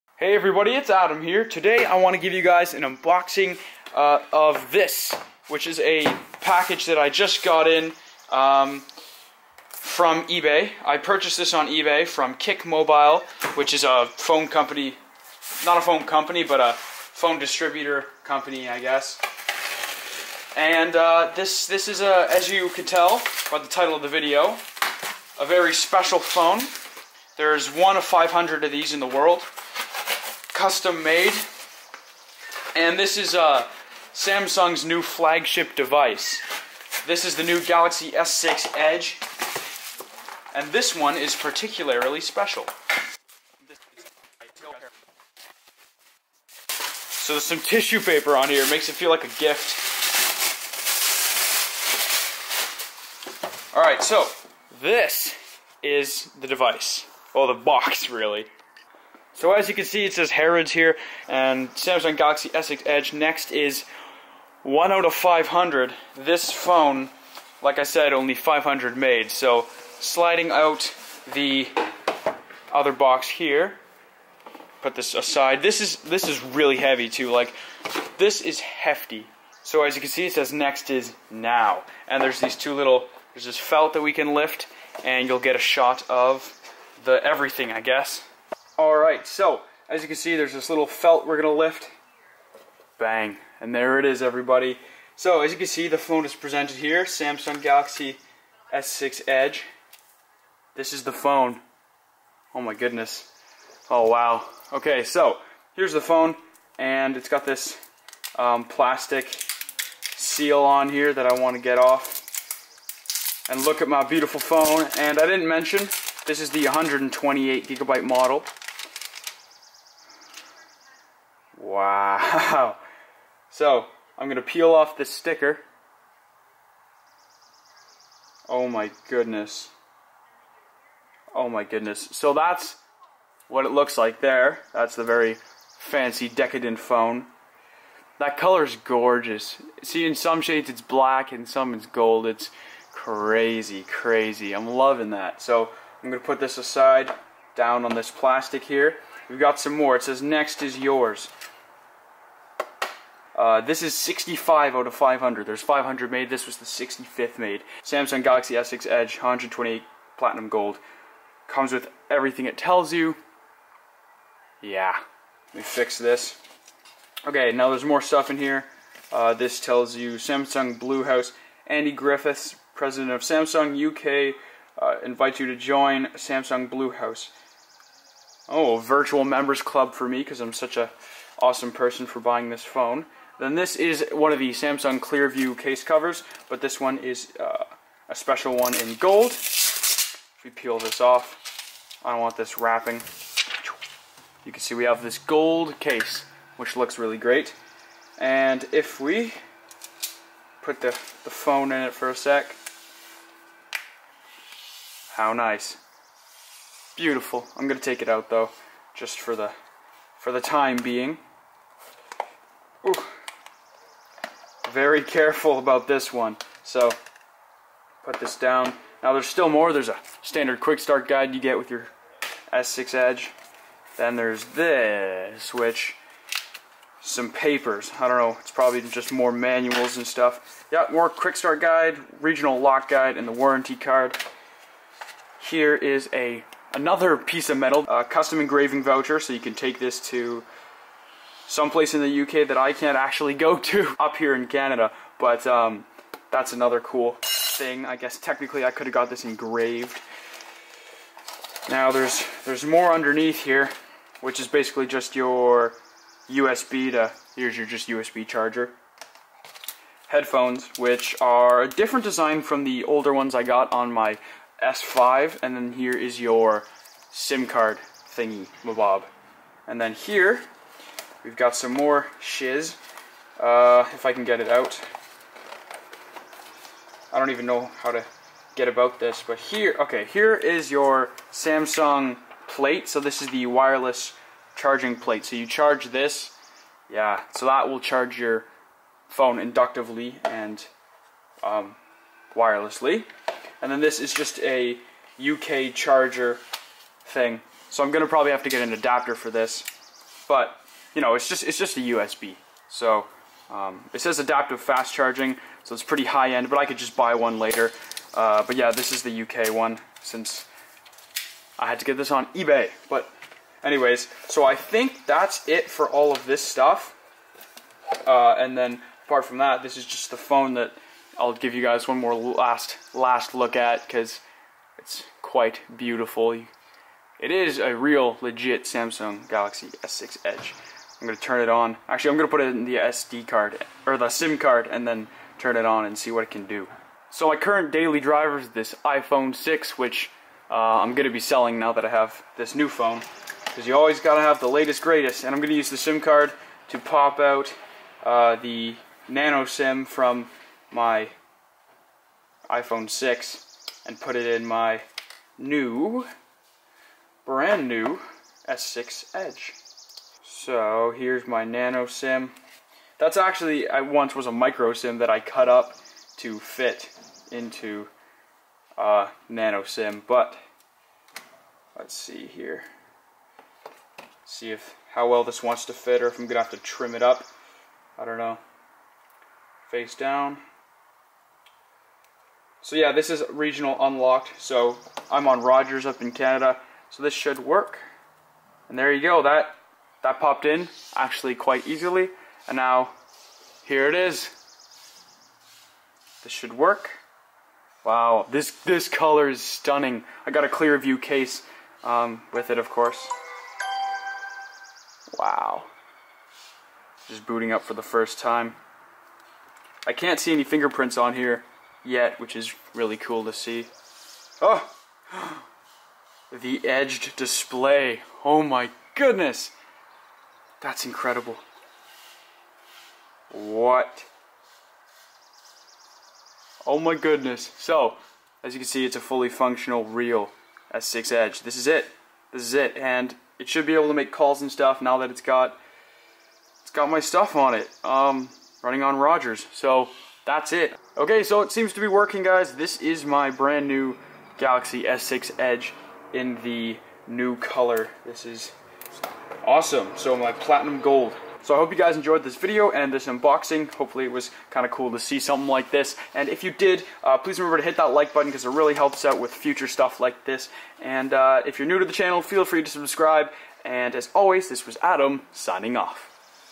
Samsung Galaxy S6 Edge 128gb Gold Platinum Limited Edition Unboxing!!!
Hey guys, today I am showing you the limited edition new samsung galaxy s6 edge 128 gb in gold platinum. There are only 500 of these phones in the world! This one is #65/500. I hope you enjoy!

‣SEND MAIL TO:
775 Strand Blvd.
Kingston, Ontario
K7P2P0
Canada